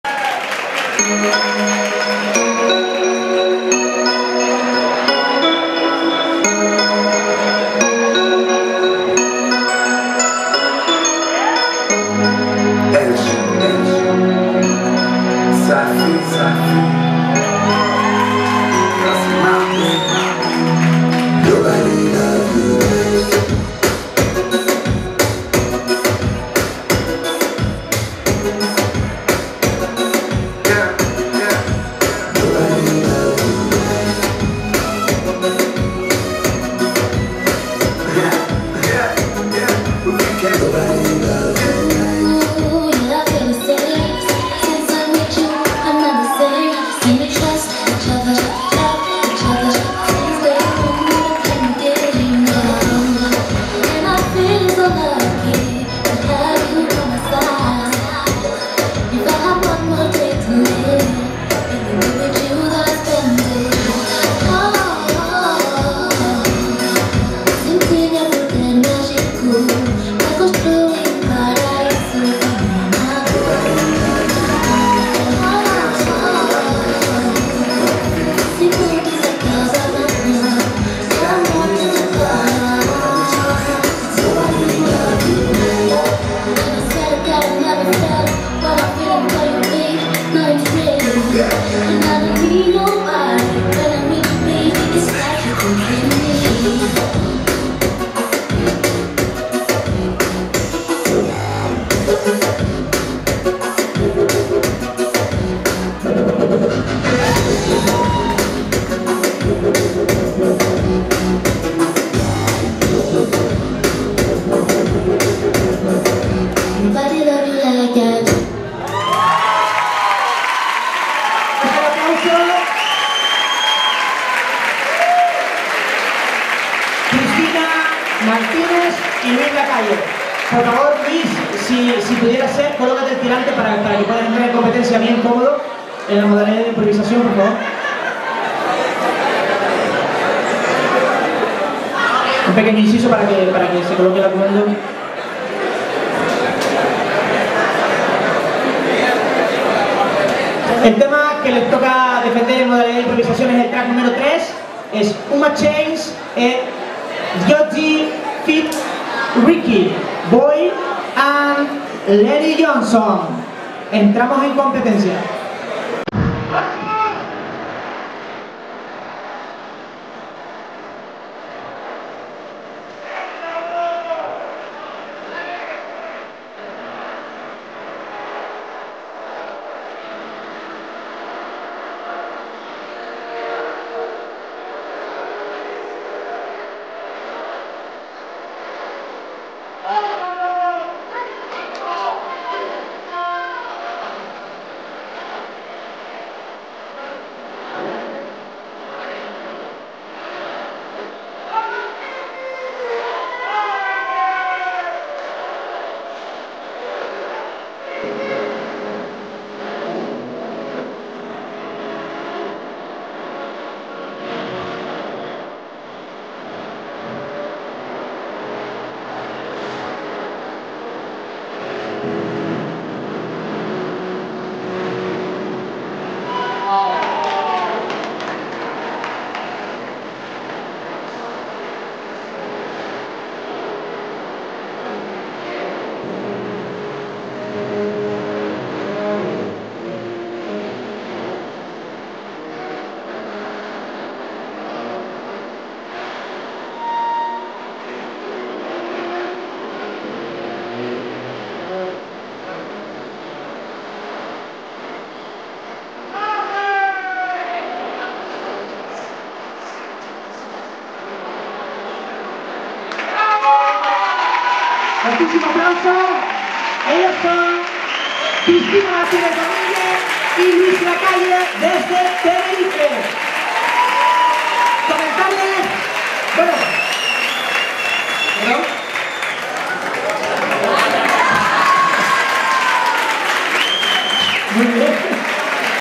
Cristina y Luis - DANZATLÁNTICA 2015 Kizomba
Resumen del certamen celebrado el día 18 de Abril con las modalidades de Bachata Amateur y Kizomba Open.
El 16 de Mayo será la final insular de Bachata modalidad Open y el próximo 23 de Mayo se celebrará la final insular de Salsa modalidad Open.